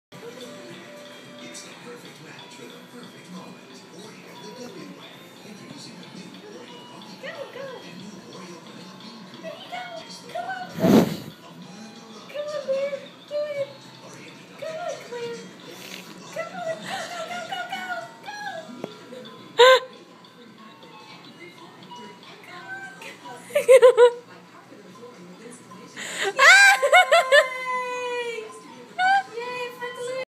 Rolling over
Claire beat rolls for the first time!!!